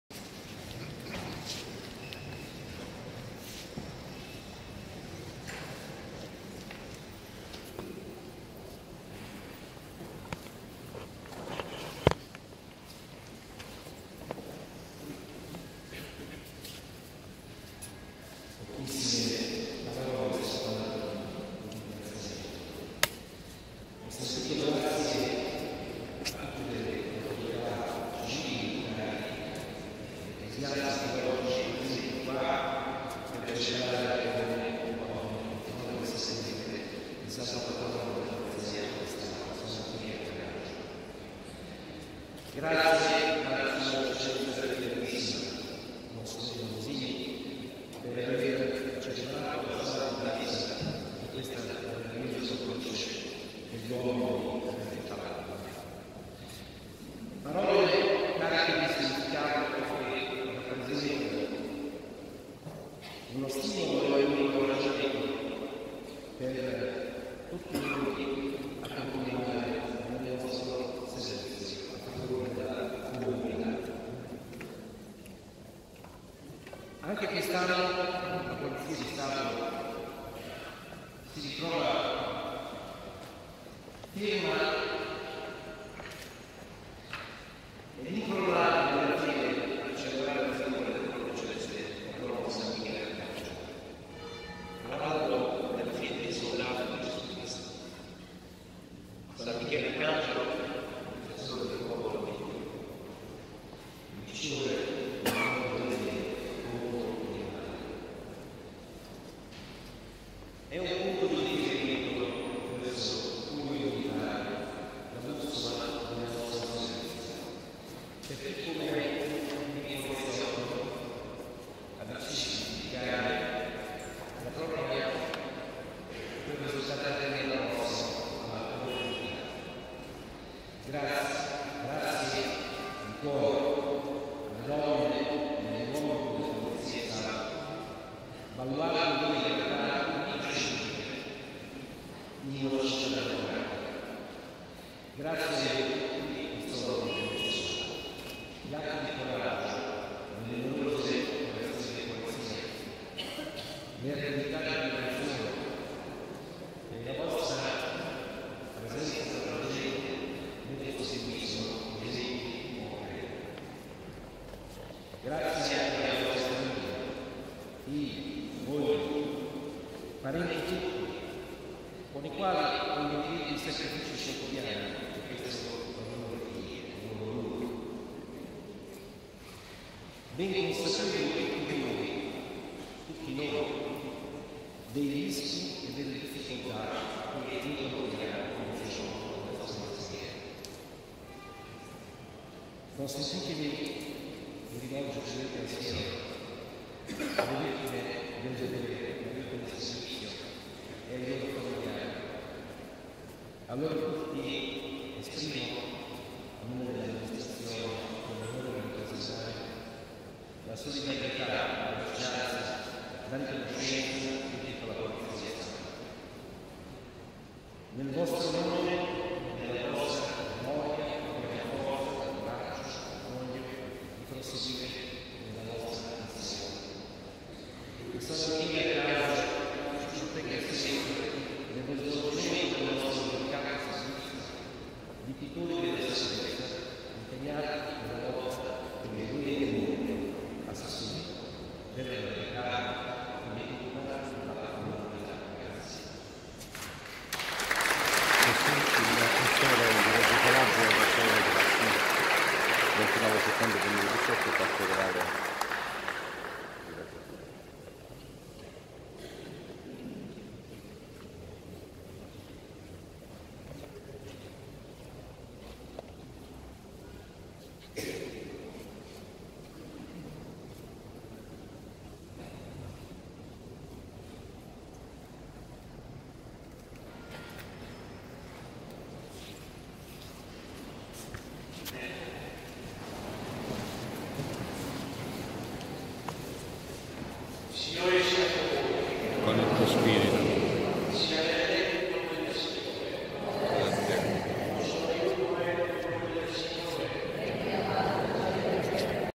L'intervento in cattedrale del Questore Raffaele Grassi In occasione della festa del santo patrono d
Reggio Calabria 29 settembre 2018 l'intervento in cattedrale del Questore di Reggio Calabria Raffaele Grassi In occasione della festa del santo patrono della polizia di stato in onore di San Michele Arcangelo

alle ore 9.30, presso il Duomo Sua Eccellenza l’Arcivescovo di Reggio Calabria, Monsignor Giuseppe Fiorini Morosini celebrerà una messa per la Festa di San Michele Arcangelo, Patrono della Polizia di Stato. Nel corso della manifestazione “Family Day”, in Piazza Duomo dalle ore 10.00 alle ore 18.00, la Polizia di Stato presenterà il “ForensicFullback”, il nuovo mezzo operativo assegnato al Gabinetto Regionale della Polizia Scientifica per la Calabria, in dotazione alle squadre impegnate nei sopralluoghi della scena del crimine. Ai ragazzi che interverranno alla presentazione verrà consegnato un “diploma di investigatore scientifico per 1 giorno”